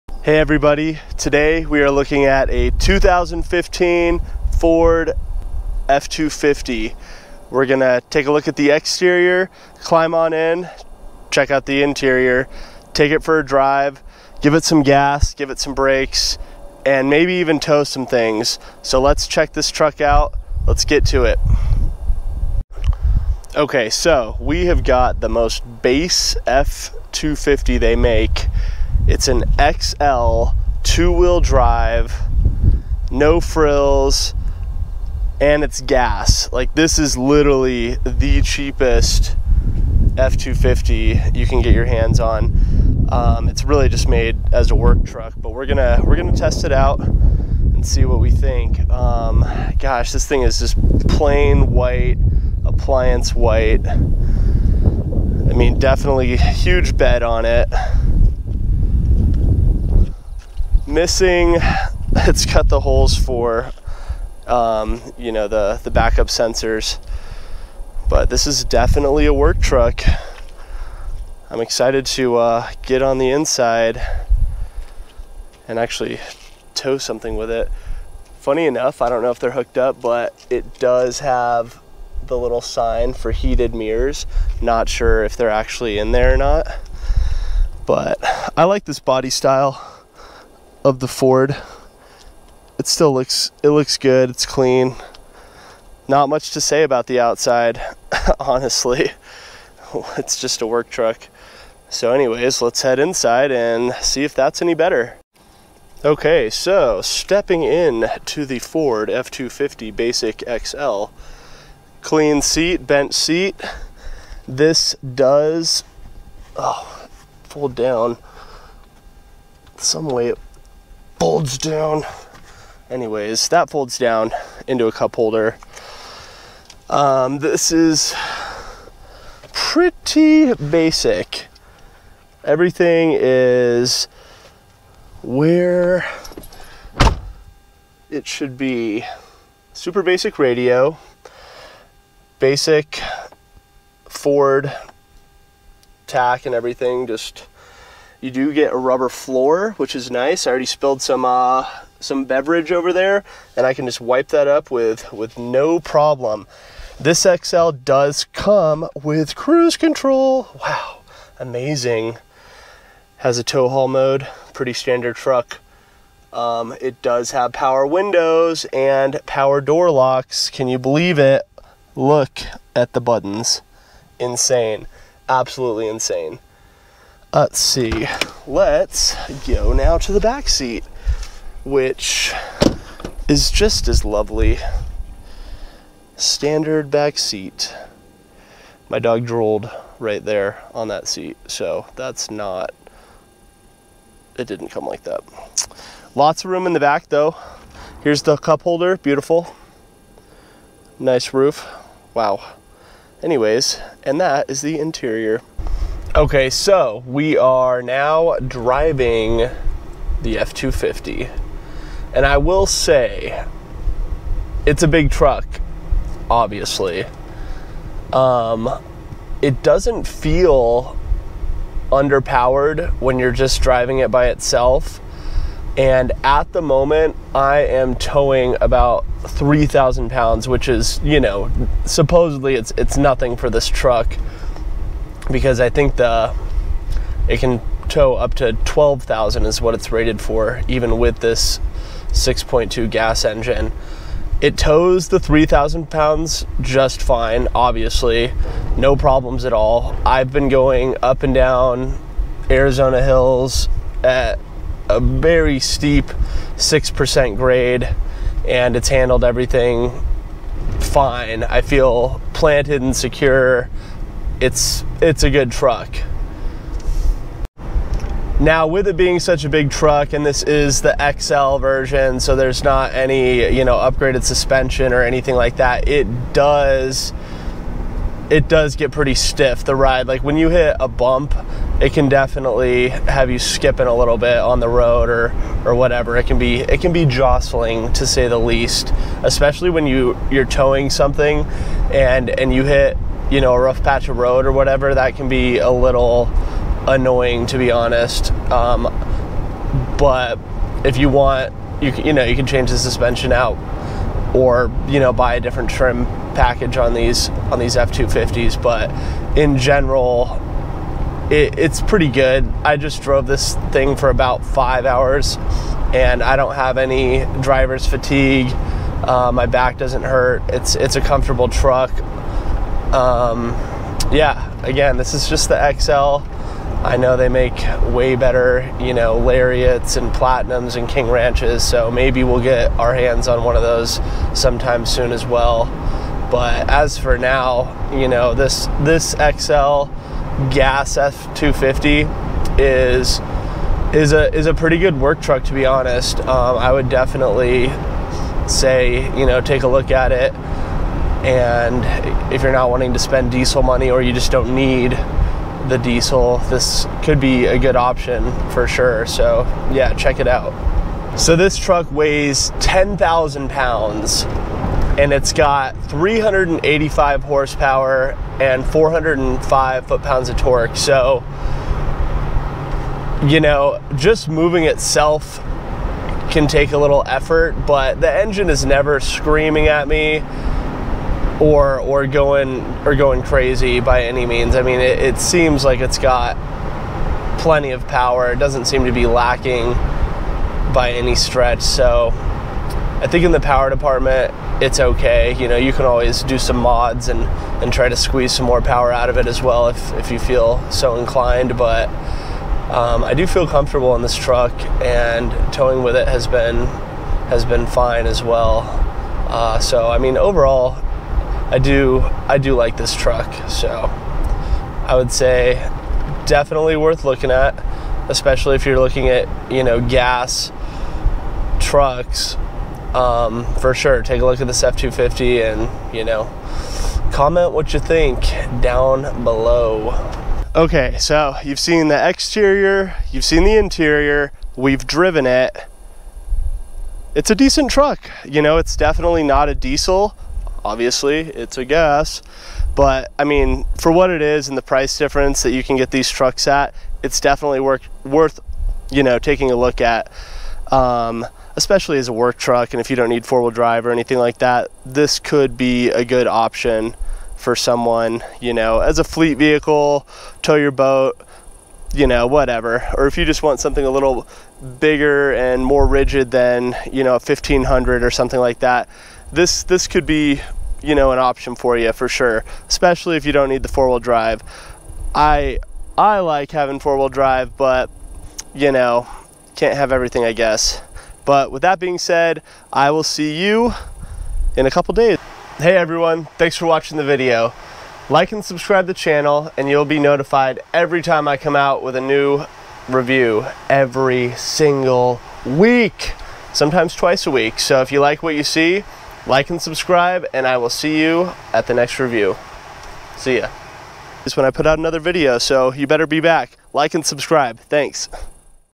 Ford F250 2015 - Truck Review - Is the most base truck worth a look?
Today we are looking at this 6.2 gas engine F250. Check out the exterior, interior, drive it and see what we think!

New vehicle reviews coming out every week.